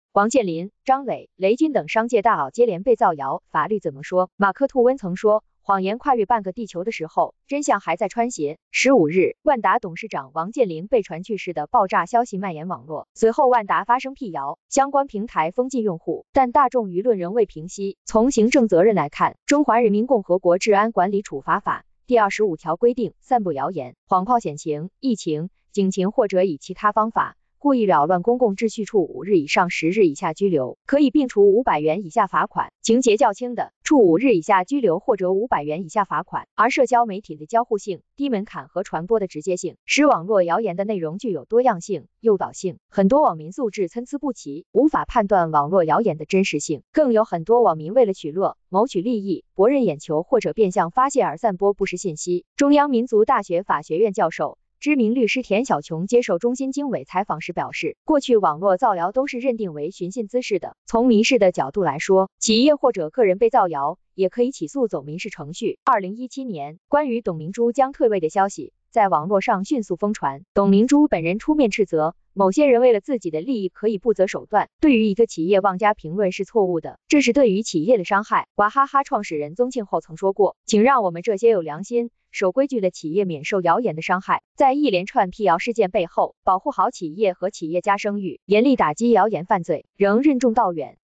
王健林、张磊、雷军等商界大佬接连被造谣 法律怎么说？
马克·吐温曾说：“谎言跨越半个地球的时候，真相还在穿鞋”。15日，万达董事长王健林被传去世的爆炸消息蔓延网络，随后万达发声辟谣，相关平台封禁用户，但大众舆论仍未平息。无独有偶，11月16日，苏宁辟谣“破产传闻”；今年8月，高瓴辟谣创始人张磊被边控传闻；当月，雷军发朋友圈辟谣“被投资人训话”传闻。7月，鸿星尔克董事长发声辟谣“快倒闭”“诈捐”；6月，银河基金辟谣董事长出轨女下属传言；2月，华为辟谣轮值董事长徐直军离职传闻……围绕企业家、企业的谣言接连出现，为何这种低成本的造谣会蔓延网络？造谣者违反哪些法律法规，需要付出多大代价？在事件背后，我们应当反思的是什么？谣言四起日前，67岁的万达集团董事长王健林被传于11月4日下午病逝，对此，万达集团官网直接发布一则王健林主持召开集团高层会议的照片，力破谣言。消息显示，11月15日，万达集团董事长王健林主持召开万达集团创新工作推进会，落实“作品为王、创新无限”要求，做好集团作品创新、管理创新、经营模式创新工作，年底评选集团创新大奖。王健林现身会议，无疑堵上了网络造谣者的嘴。据报道，万达方面表示，对于王健林去世的传言是谣言，已经报警。来源：万达集团官网当日，今日头条发布处罚公告称，11月14日21时21分，微头条用户@身拥大海2捏造某知名商业人士去世消息。22时02分，头条内容质量团队发现并下架了该谣言。经核实，截至删除时，相关内容被展现56次，阅读13次，平台已对该用户永久封禁。王健林被造谣一事还未平息，16日一早，苏宁集团发布声明称，“苏宁易购12月底宣布破产”为谣言，公司经营一切正常。苏宁易购已向公安机关报案，对造谣者将依法追究法律责任。不止万达、苏宁深陷被造谣的困扰，今年8月初，网传高瓴创始人张磊被边控。对此，高瓴相关人士向中新经纬表示，网络所传关于高瓴高管个人的信息严重不实，高瓴已向警方报案并保留一切合法追诉权益。6月23日，银河基金董事长被传出轨女下属，对此，银河基金公司发布公告称，网上传播的关于诽谤我司董事长及员工的微信聊天记录系恶意伪造。公司及当事人已向公安机关报案。法律责任几何？“造谣一张嘴，辟谣跑断腿”，万达就王健林去世谣言报警 的话题15日持续挂在微博热搜上，截至发稿话题阅读量近7亿，讨论超1万条。事件造成的传播之广可见一斑。微博截图造谣者应承担哪些法律责任？北京声驰律师事务所翟钦蕊律师告诉中新经纬，从民事责任来看，《民法典》第995条规定：人格权受到伤害的，受害人有权依照本法和其他法律的规定请求行为人承担民事责任。受害人的停止侵害、排除妨碍、消除危险、消除影响、恢复名誉、赔礼道歉请求权，不适用诉讼时效的规定。从行政责任来看，《中华人民共和国治安管理处罚法》第25条规定：散布谣言，谎报险情、疫情、警情或者以其他方法故意扰乱公共秩序处5日以上10日以下拘留，可以并处500元以下罚款；情节较轻的，处5日以下拘留或者500元以下罚款。从刑事责任来看，《刑法》第246条规定：以暴力或者其他方法公然侮辱他人或者捏造事实诽谤他人，情节严重的，处三年以下有期徒刑、拘役、管制或者剥夺政治权利。中南财经政法大学数字经济研究院执行院长盘和林对中新经纬表示，“网络造谣蔓延，是因为人类喜欢八卦的天性，人类学家认为我们人类对抽象事物能够形成概念，并乐于此道。而互联网传播谣言最重要的推动力，是互联网传谣存在隐蔽性，一方面传谣者往往认为法不责众，另一方面传谣者往往认为躲在网络背后，自己是安全的。”北京中银律师事务所杨保全律师接受中新经纬采访时也指出，我国对网络言论的监管尚属薄弱，不管是从手段上还是技术上正处于积极探索阶段。而社交媒体的交互性、低门槛和传播的直接性，使网络谣言的内容具有多样性、诱导性，很多网民素质参差不齐，无法判断网络谣言的真实性，更有很多网民为了取乐、谋取利益、博人眼球或者变相发泄而散播不实信息。留下什么反思？值得一提的是，王健林被造谣背后，市场上有消息称，“万达商管已向港交所递交了IPO材料，正在谋求上市，此次谣言疑有人恶意阻止万达上市。”但此说法未得到万达方面证实，此外，案件最终结论还有待警方声明。往大了说，企业家在企业的生存和发展过程中起着至关重要的作用，在社会上也有极强的影响力。往小了说，造谣对个人和企业的伤害都是无可弥补的，会对身心造成恶劣影响。中央民族大学法学院教授、知名律师田小穹接受中新经纬采访时表示，过去网络造谣都是认定为寻衅滋事的，从民事的角度来说，企业或者个人被造谣也可以起诉走民事程序。“其实造谣这件事情对被骚扰者生活上的影响是可以理解的，它触动了两个问题，一是严重侵犯被造谣者的合法权益，影响生活的安宁，同时它也扰乱了社会秩序。”事实上，商界大佬被造谣已屡见不鲜，格力电器董事长董明珠、福耀玻璃创始人曹德旺就曾经多次卷入谣言。2017年，关于董明珠将“退位”的消息在网络上迅速疯传，董明珠本人出面斥责：“某些人为了自己的利益可以不择手段，对于一个企业妄加评论是错误的，这是对于企业的伤害。”2016年底，福耀玻璃在美国建厂一度衍生出“别让曹德旺跑了”的话题，最后本尊出面回应说“我没跑”。董明珠资料图娃哈哈创始人宗庆后曾说过，请“让我们这些有良心、守规矩的企业免受谣言的伤害。让企业家和企业都能专心把自己的事情做好，而不是忙着辟谣。”2017年，中共中央、国务院出台了文件《关于营造企业家健康成长环境弘扬优秀企业家精神更好发挥企业家作用的意见》，提出着力营造依法保护企业家合法权益的法治环境，营造积极向上的舆论氛围，展示优秀企业家精神，凝聚崇尚创新创业正能量，营造尊重企业家价值、鼓励企业家创新、发挥企业家作用的舆论氛围。在一连串辟谣事件背后，保护好企业和企业家声誉，严厉打击谣言犯罪，仍任重道远。